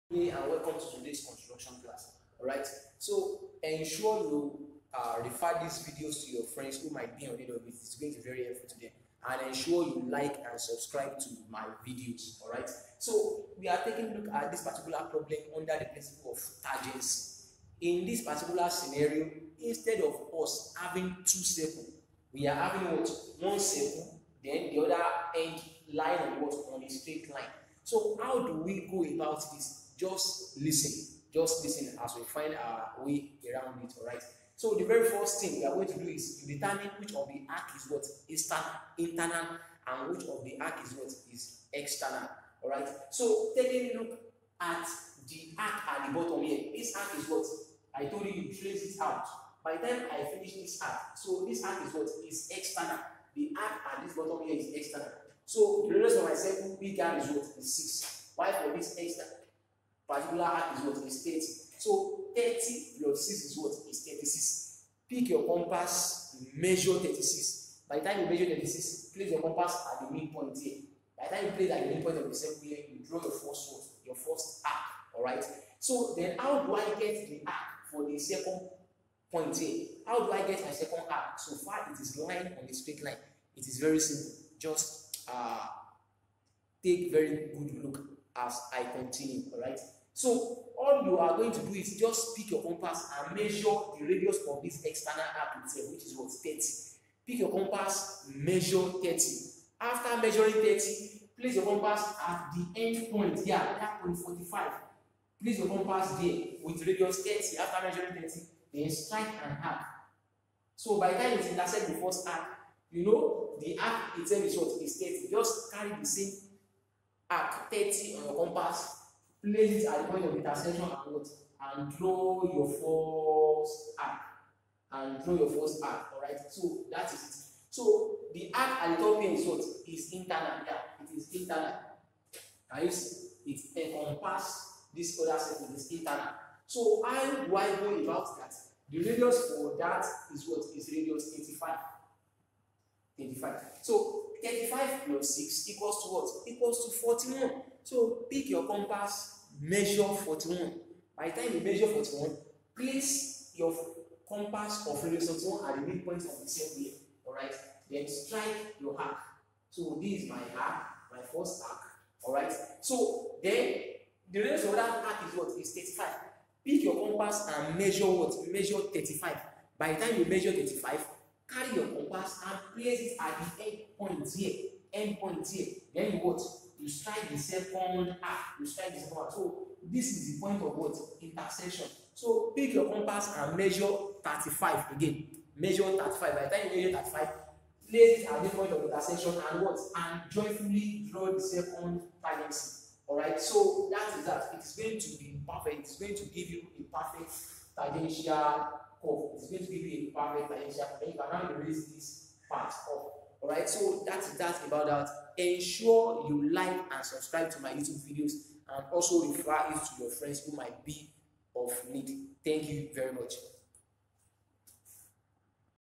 TANGENCY part 2 in | Technical drawing | Engineering drawing
How to construct curve TANGENCY using the three principles of addiction, subtraction, and TANGENCY on a straight line.
Check the links below for Solved examples